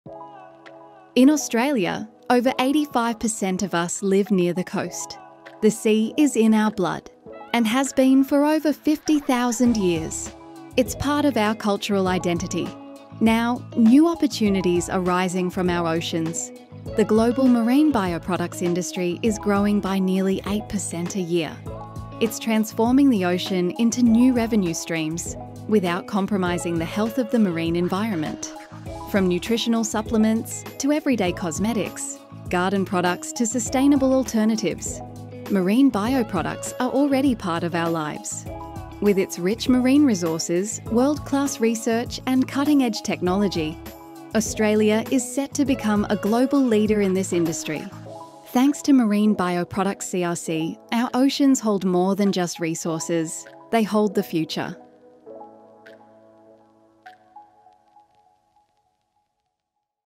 Marine Bioproducts
New opportunities are rising from our oceans. From medicines to everyday beauty products, building materials to sustainable alternatives, marine bioproducts are already part of our lives.

Dive in and discover how at Marine Bioproducts CRC, we’re supporting industry with new income streams from our seas